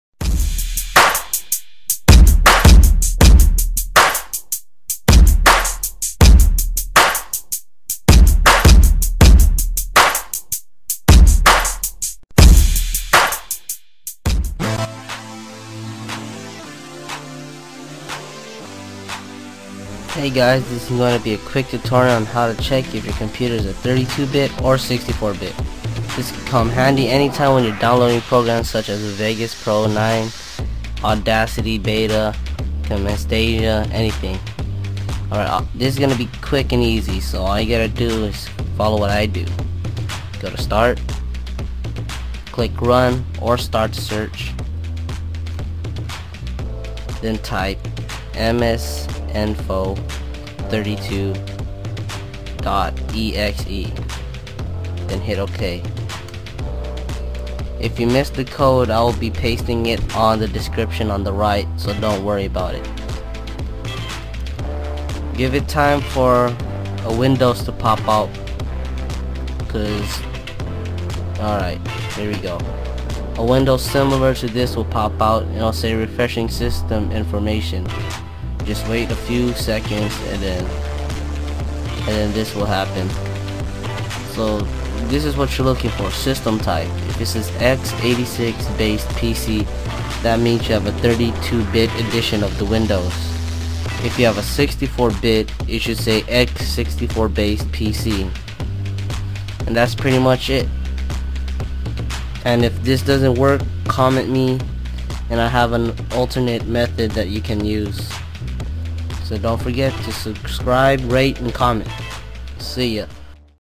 How to find if your computer is 32 or 64bit
How to check if your computer is an 34 bit or 64bit
This can come handy when downloading programs and they ask you
34bit download or 64bit.

Code: msinfo32.exe

For 32-bit editions of Windows, the value of the System Type item is x86-based PC.
For 64-bit editions of Windows, the value of the System Type item is x64-based PC.